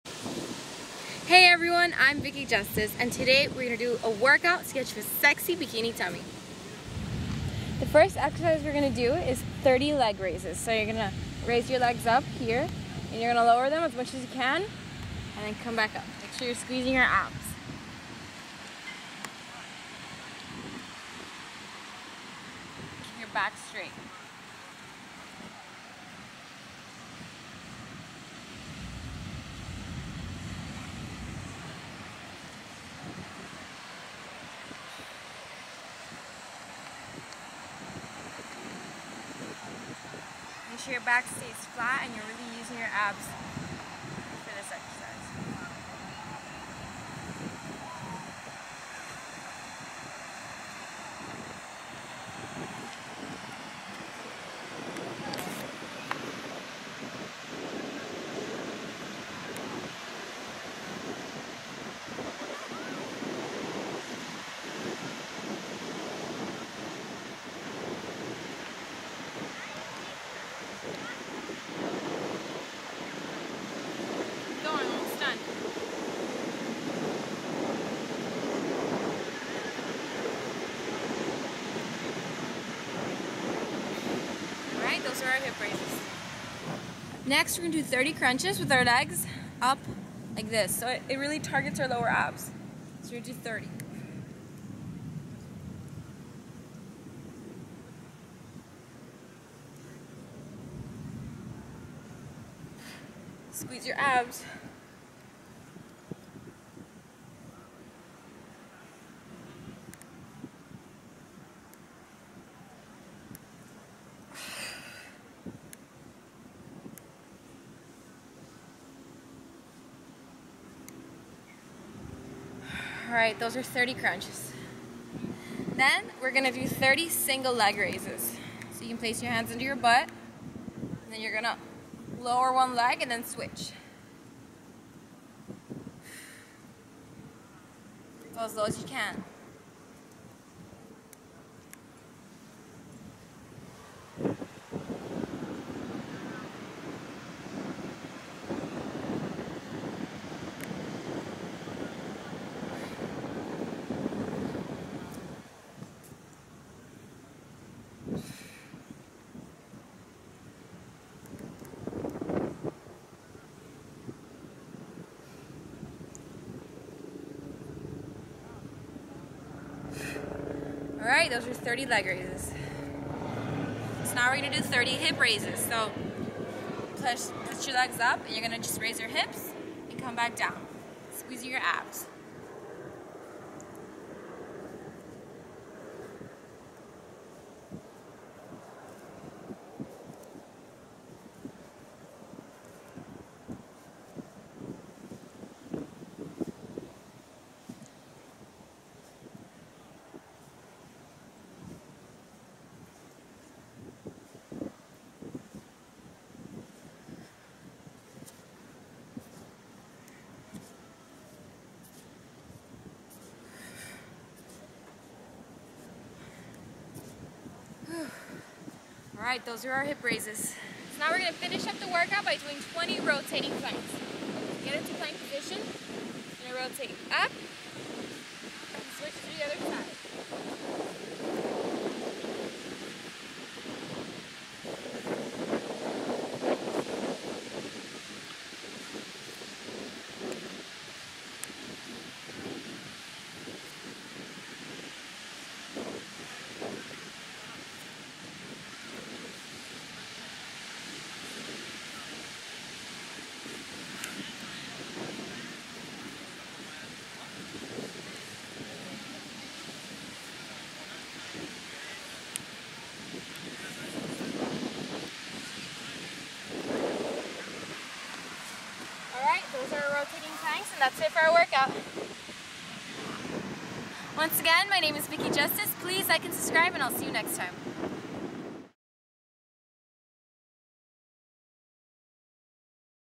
Sexy Bikini Tummy Workout!
Here's a stomach workout you can do at home, the beach, or the gym that will help you get sexy flat abs. And it takes less then 5 minutes and can be done every day!